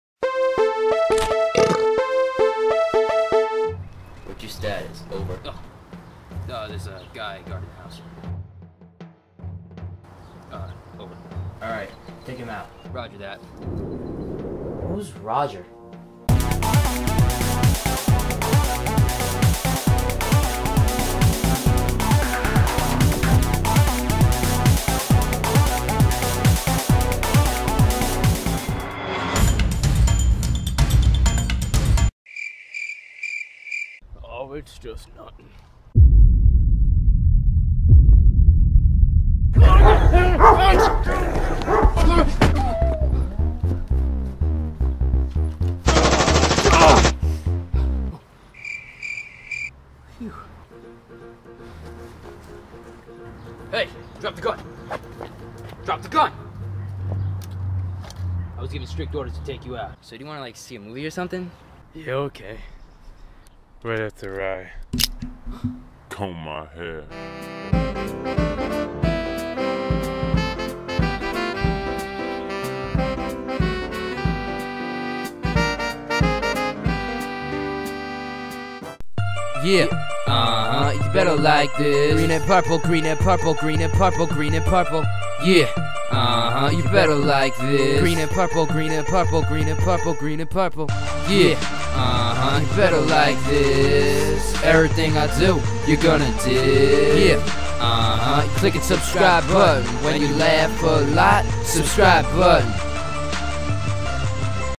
The Miscommunication
Military Op. gone wrong. (Bloopers will be up in a few days, subscribe to the second channel to see) Share the video and stuff cause I went out to get military stuff and what not. Show the love